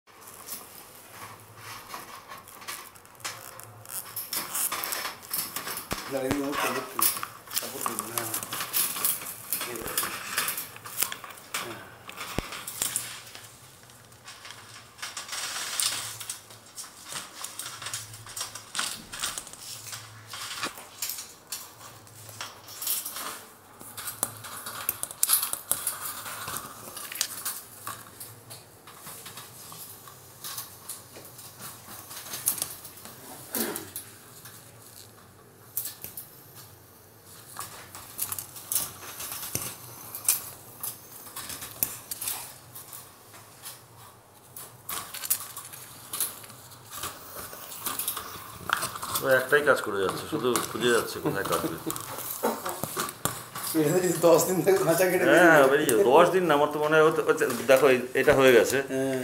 Rescued wounded parrot trying to cut the cage to escape.
Ultimately cutting The cage this parrot escaped within an hour.